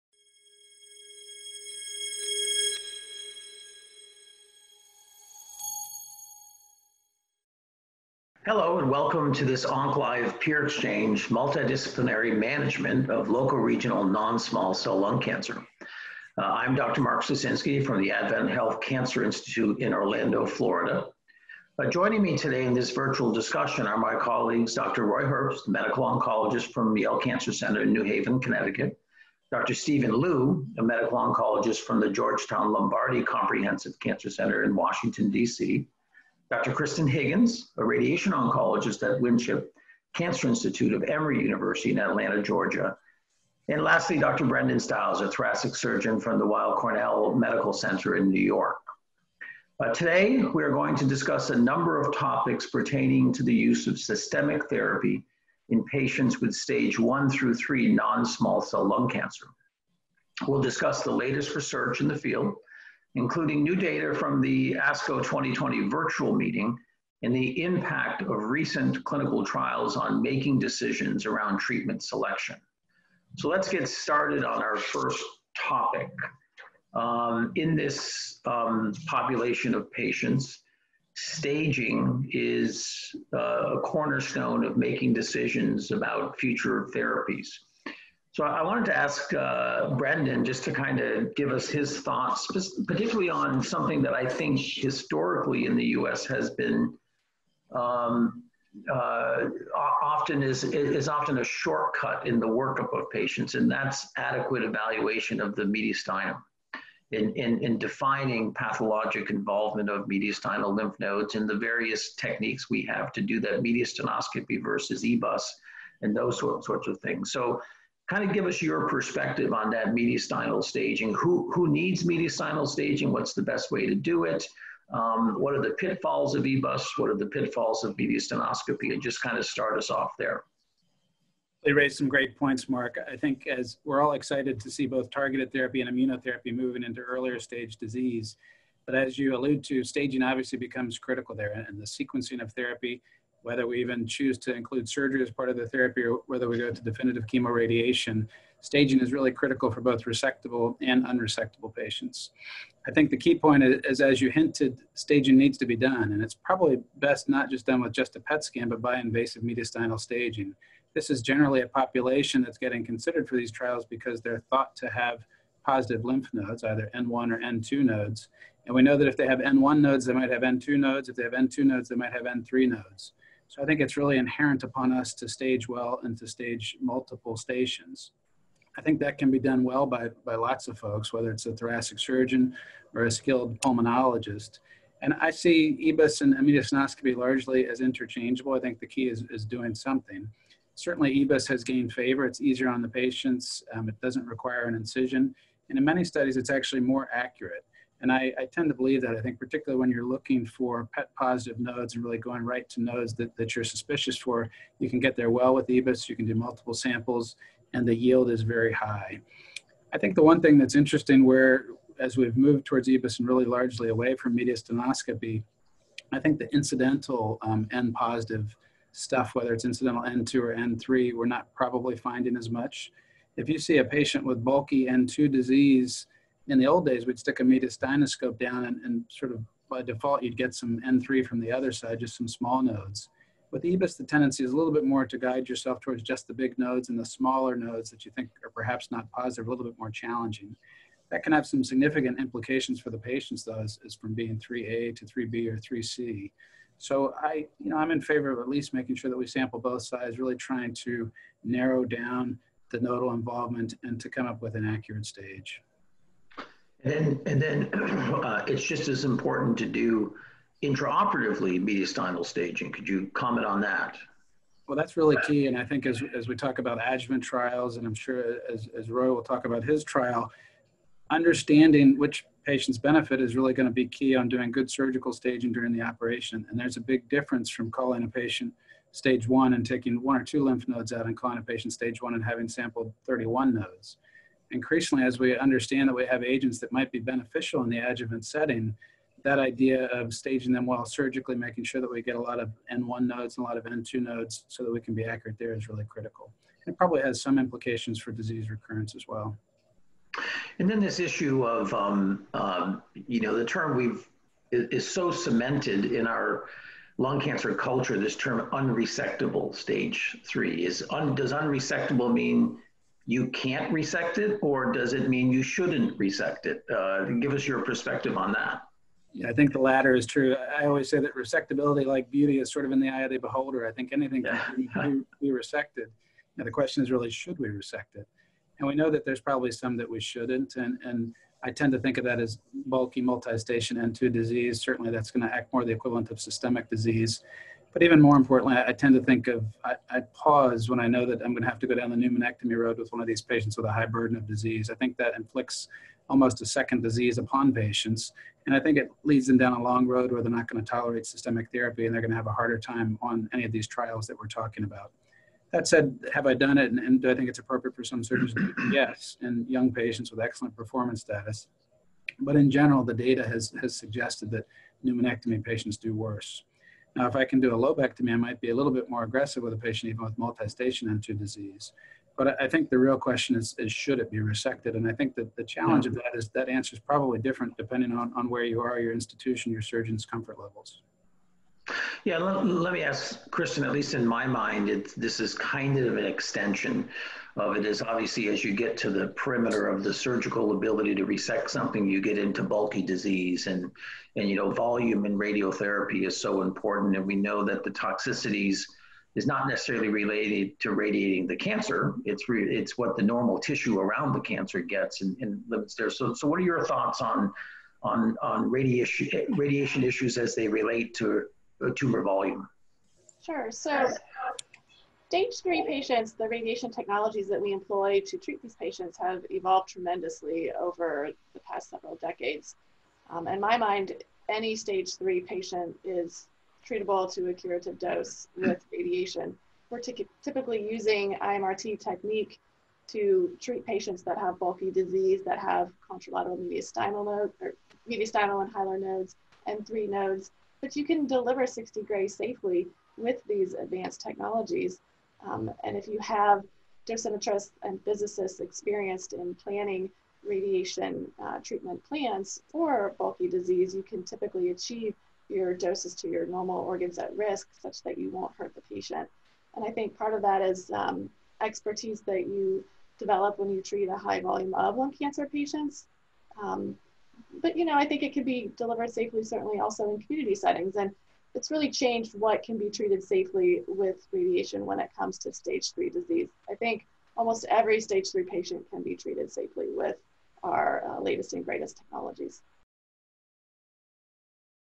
Mediastinal Staging in Non–Small Cell Lung Cancer
Brendon Stiles, MD, shares his perspective on mediastinal staging using EBUS vs mediastinoscopy in the diagnosis of non–small cell lung cancer.